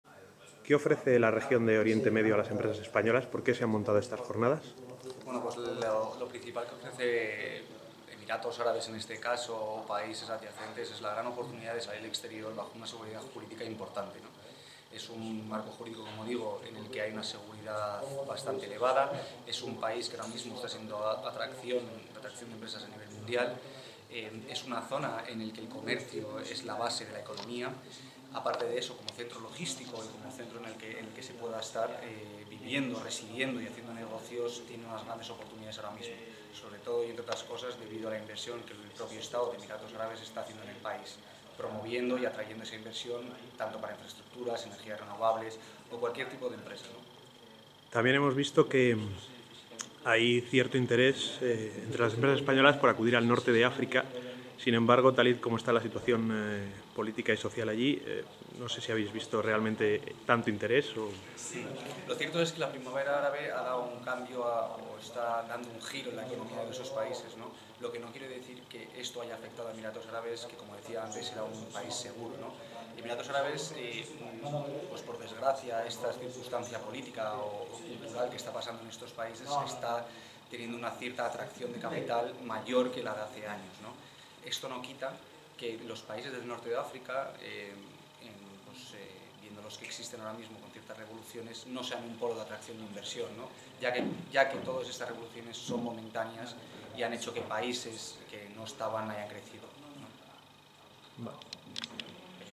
Más de un centenar de pymes españolas buscan expandirse en Oriente Medio
El Grupo RLD ha logrado reunir a más de cien pymes españolas en unas conferencias que invitan a expandirse por Oriente Medio.